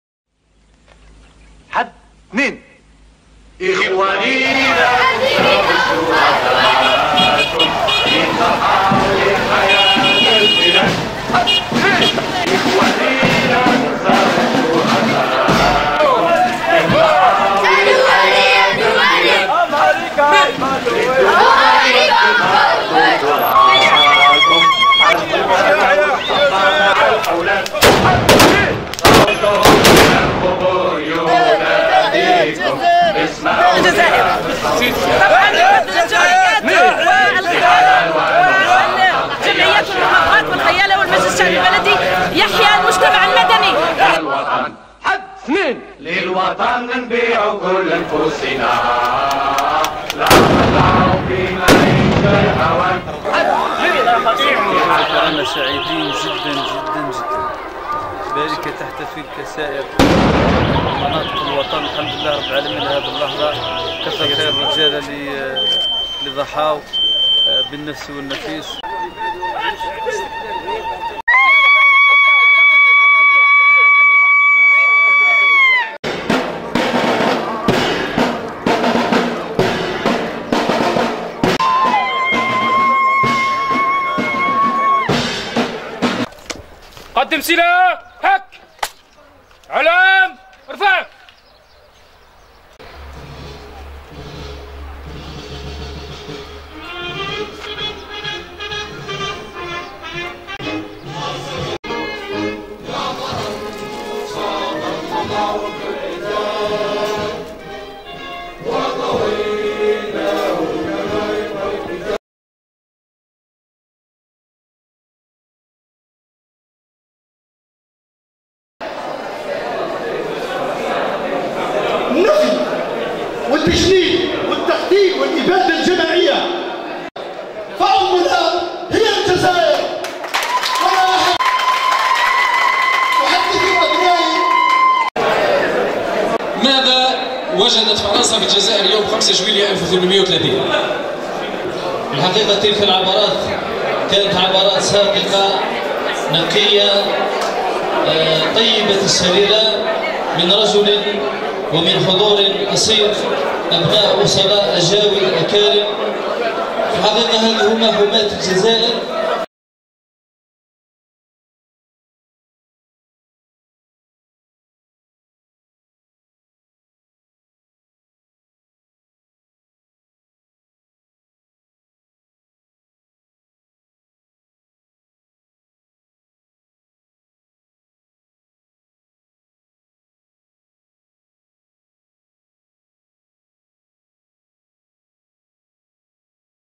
هكذا احتفلت بلدية بريكة بباتنة بالذكرى الـ61 لعيد الاستقلال
5000
فيسبوك :